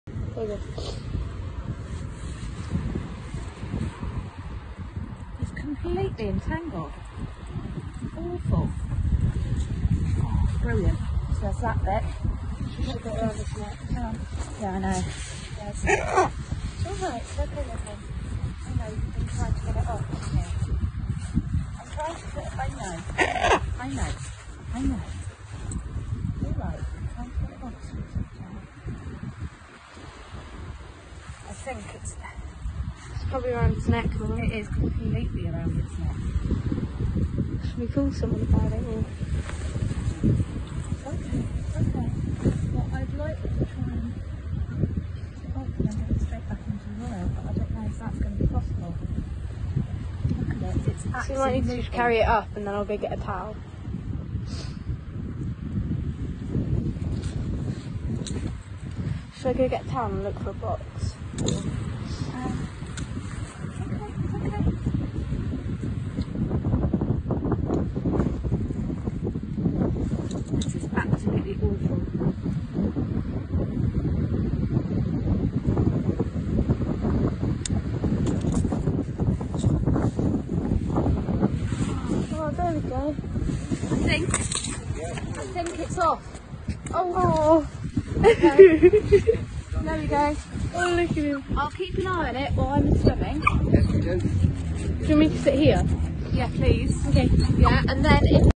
Oreo the guillemot being rescued in Guernsey wrapped in fishing line now safe at the GSPCA VT1
For advice on what to do if you find injured wildlife please check out or to set up a regular donation please check out these GSPCA